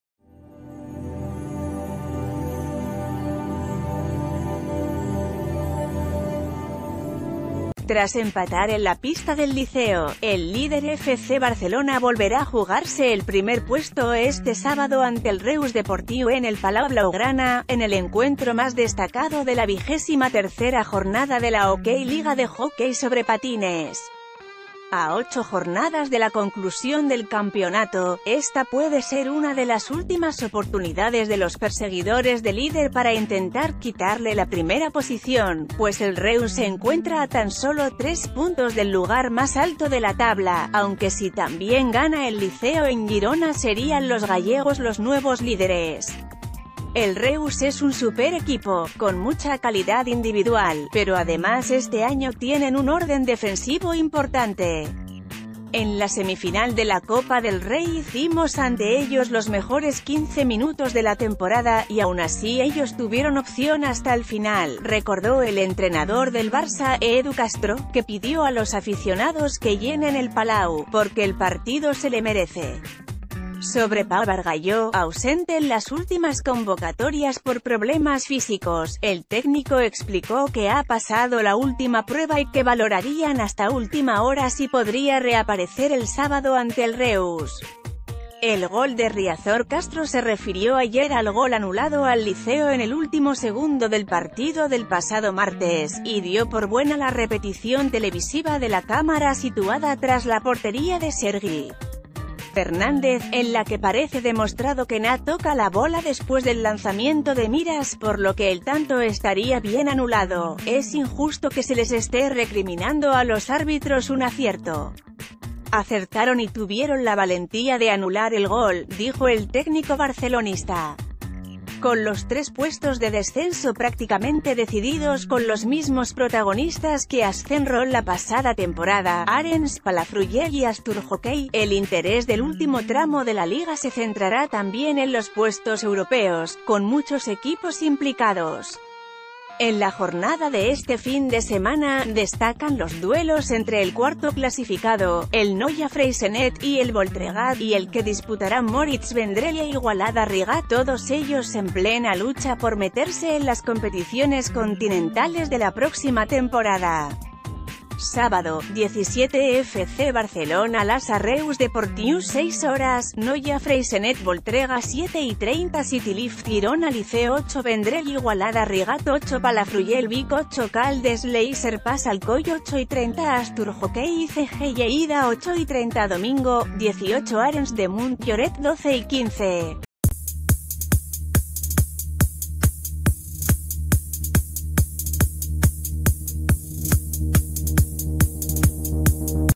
Noticias de última hora | El Barça se juega seguir lider en solitario ante el Reus Deportiu, tercero
Tras empatar en la pista del Liceo, el líder FC Barcelona volverá a jugarse el primer puesto este sábado ante el Reus Deportiu en el Palau Blaugrana, en el encuentro más destacado de la vigésima tercera jornada de la OK Liga de hockey sobre patines. A ocho jornadas de la conclusión del campeonato, esta puede ser una de las últimas oportunidades de los perseguidores de líder para intentar quitarle la primera posición, pues el Reus se encuentra a tan sólo tres puntos del lugar más alto de la tabla...